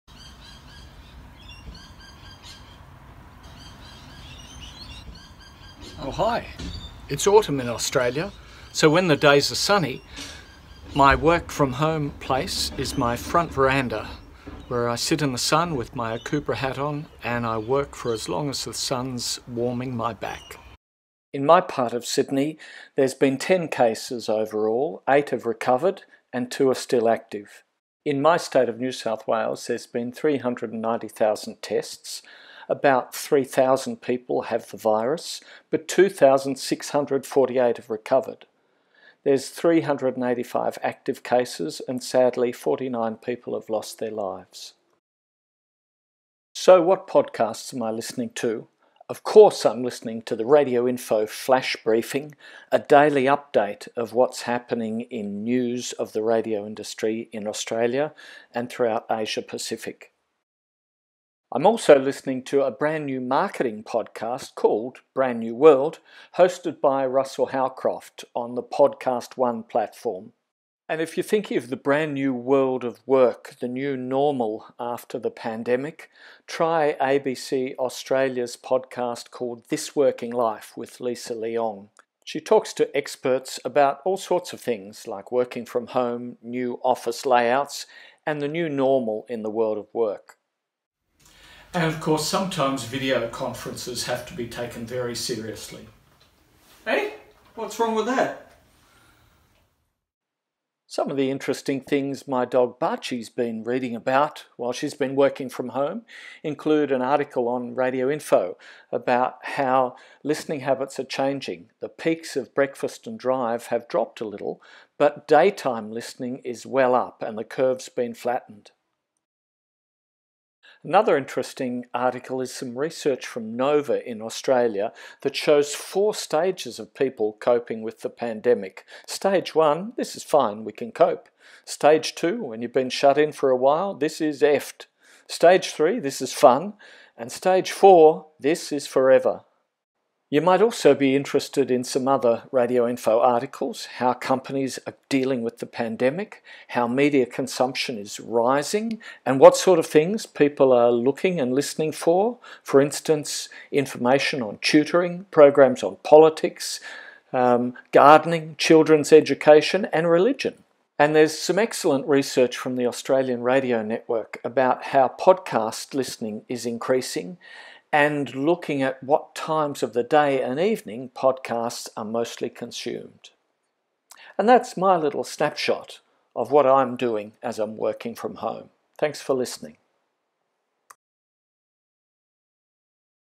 Steve Ahern Message
Steve Ahern, Publisher of radioinfo.com.au and asiaradiotoday.com tells us all about his experience working from home in Sydney, Australia during COVID-19. He also provides some great podcast and article recommendations.